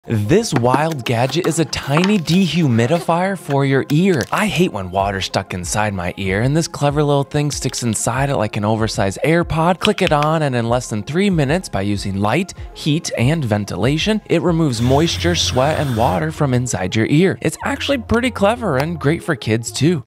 Swimmer's Ear Air Dryer! 👂💦
This mini dehumidifier fits inside your ear and helps to remove water and moisture, good for adults, kids, or even pets.

DearBuds SE (Amazon)